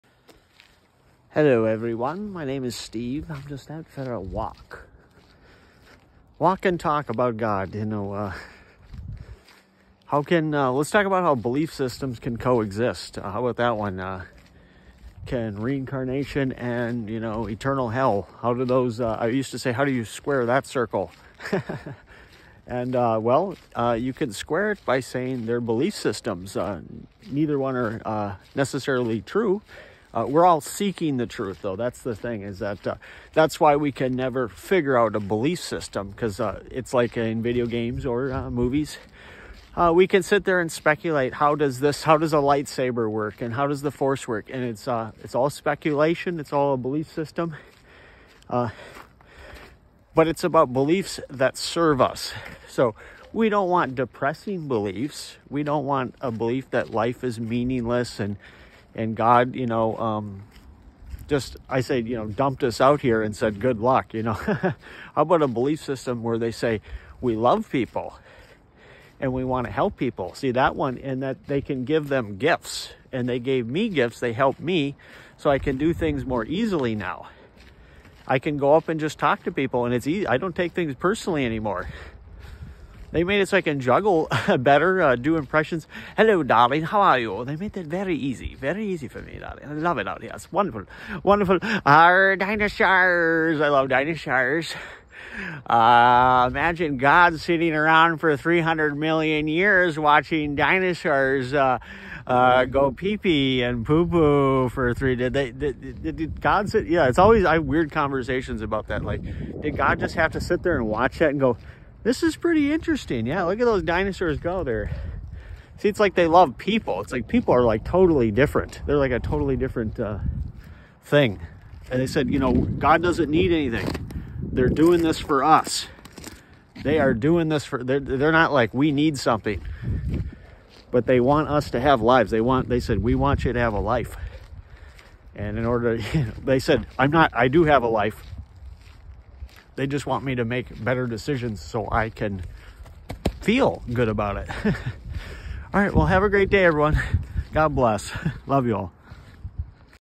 Walk and talk about God, belief systems that contradict, God makes it easier for me to do things
I talk about god belief systems. God wants us to believe in good things not depressing things they want to help us give us gifts they’re doing it for us what’s with dinosaurs? Any clue on that one? Sharing Enlightenment God. ￼